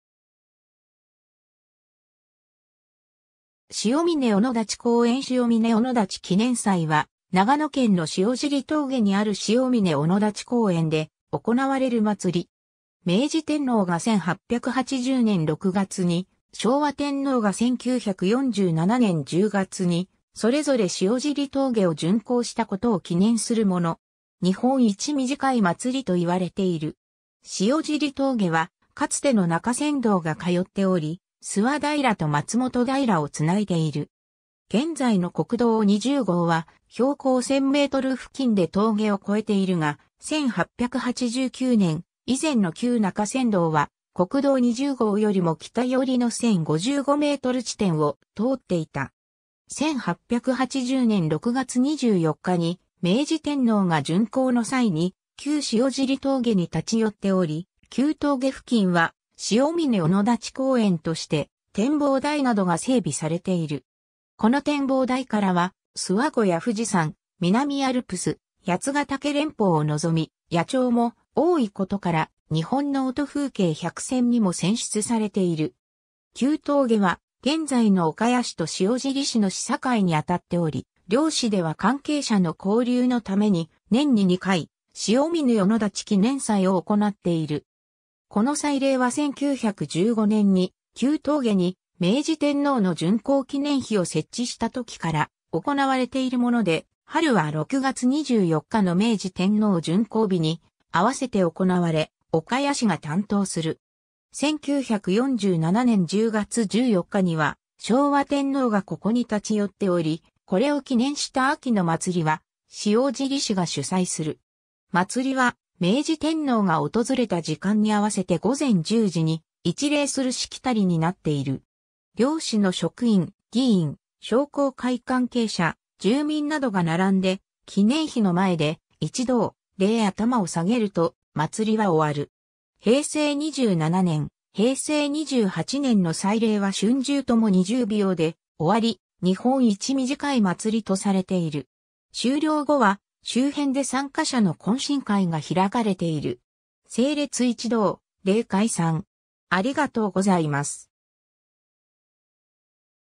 塩嶺御野立記念祭
塩嶺御野立公園 塩嶺御野立記念祭（えんれいおのだちきねんさい）は、長野県の塩尻峠（岡谷市側）にある塩嶺御野立公園で行われる祭り。
明治天皇が1880年（明治13年）6月に、昭和天皇が1947年（昭和22年）10月に、それぞれ塩尻峠を巡幸したことを記念するもの。
「日本一短い祭り」と言われている。
 塩尻峠はかつての中山道が通っており、諏訪平（現在の岡谷市）と松本平（塩尻市）を繋いでいる。
現在の国道20号は標高1000m付近で峠を越えているが、1889年（明治22年）以前の旧中山道は国道20号よりも北寄りの1055m地点を通っていた。
 1880年（明治13年）6月24日に明治天皇が巡幸の際に旧塩尻峠に立ち寄っており、旧峠付近は「塩嶺御野立公園」として展望台などが整備されている。
この展望台からは諏訪湖や富士山、南アルプス、八ヶ岳連峰を望み（「関東の富士見百景」）、野鳥も多いことから「日本の音風景100選」（塩嶺の小鳥のさえずり）にも選出されている。
 旧峠は現在の岡谷市と塩尻市の市境にあたっており、両市では関係者の交流のために、年に2回「塩嶺御野立記念祭」を行っている。
この祭礼は1915年（大正4年）に旧峠に明治天皇の巡幸記念碑を設置した時から行われているもので、春は6月24日の明治天皇巡幸日にあわせて行なわれ、岡谷市が担当する。
1947年（昭和22年）10月14日には昭和天皇がここに立ち寄っており、これを記念した秋の祭りは塩尻市が主催する。
 祭りは明治天皇が訪れた時間に合わせて午前10時に一礼するしきたりになっている。
両市の職員、議員、商工会関係者、住民などが並んで記念碑の前で「一同、礼」頭を下げると祭りは終わる。
平成27年、平成28年の祭礼は春秋とも20秒で終わり、「日本一短い祭り」とされている。
終了後は、周辺で参加者の懇親会が開かれている。
 整列 一同「礼」 解散